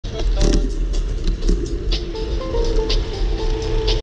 Transportistes que condueixen amb compte
En aquest cas, aquest conductor para, mira, em veu venir i m'indica repetidament que puc passar. I jo li dono les gràcies amb un gest que no es veu al vídeo.